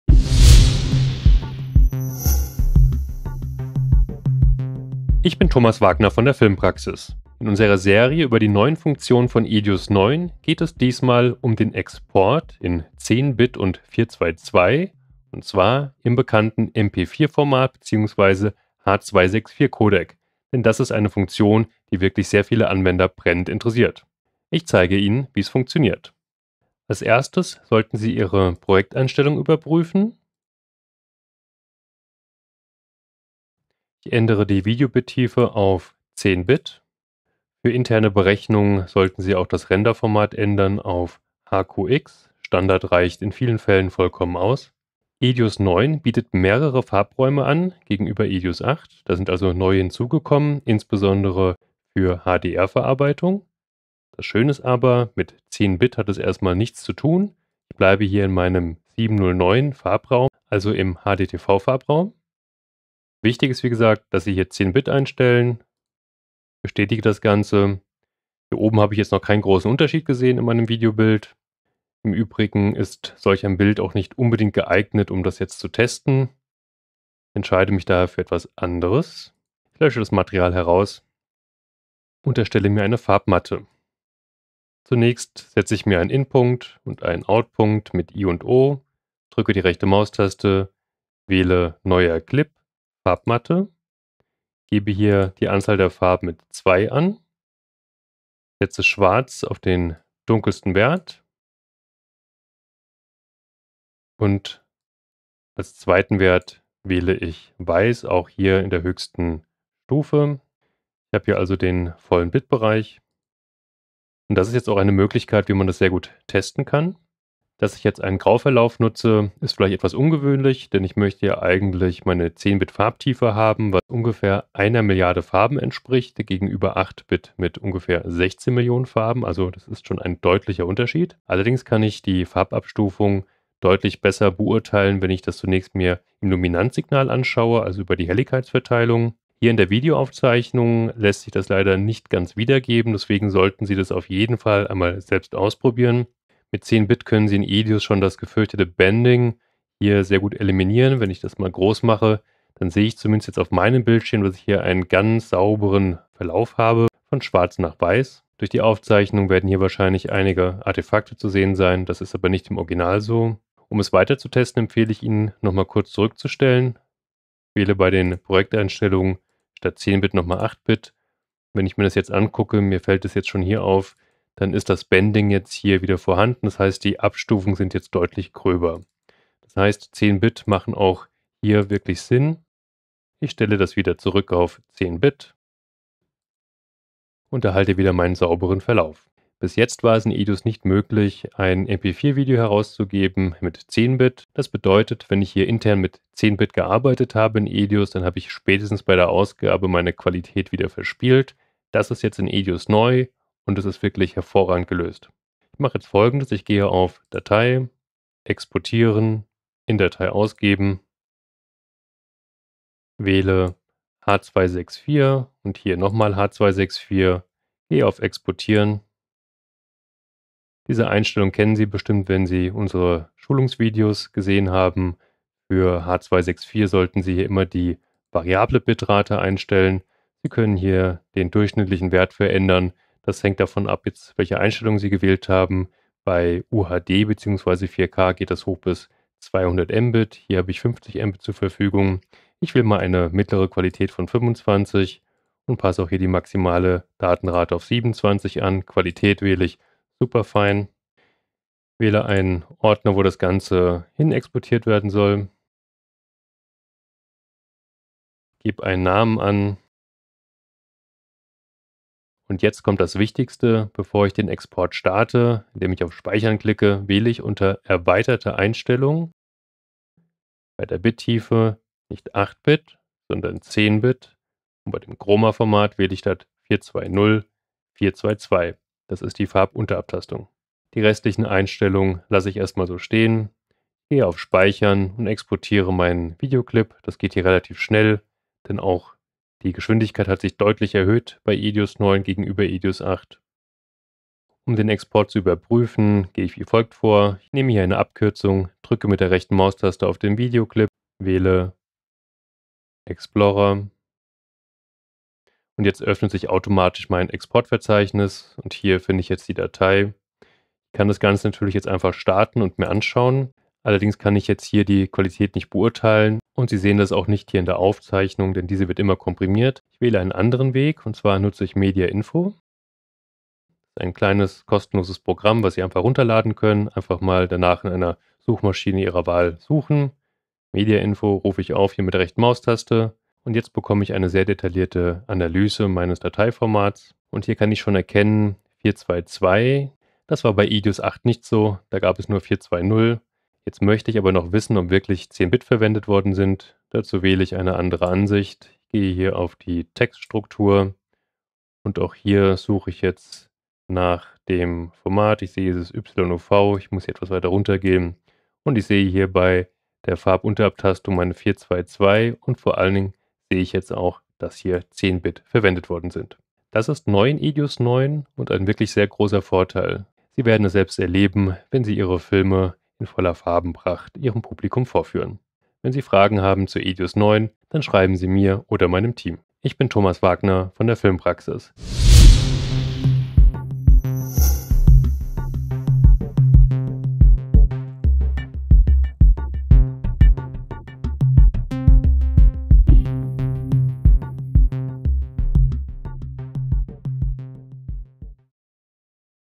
EDIUS 9 - Video-Export in MP4 (H.264) in 10 Bit in 4:2:2 (über 1 Milliarde Farben)
Nutzen Sie über 1 Milliarde Farben in Ihren Videos (und nicht nur 16 Millionen). In diesem Video erklären wir Ihnen den Export in MP4 (H.264/AVC) mit 4:2:2 (Farbunterabtastung).

Dieses Video-Training ist Teil unserer Serie "Die neuen Funktionen in EDIUS 9".

Möchten Sie noch mehr Profi-Wissen?

Haben Sie Fragen?
Dann schreiben Sie uns. Kontaktdaten finden Sie auf unserer Webseite